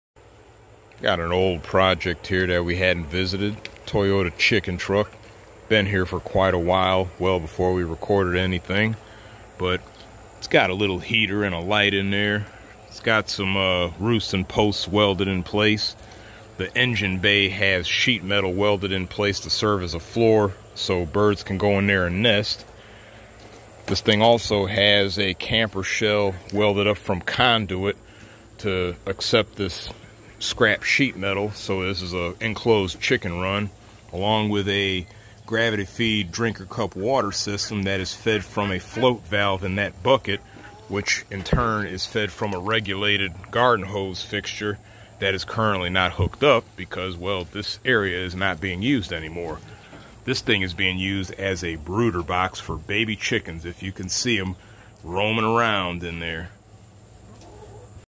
The Toyota Chicken Truck.
Here's a project we haven't visited on camera as it has been around before we started recording.  The Toyota Chicken Truck, made from an old Toyota Pickup, has served us for many years but as time passed the coop has been relegated to broader box duty.  I have some automotive plans for this truck's frame as I have another frame swap project in the works.  I also have plans for the cab as far as chicken coops go.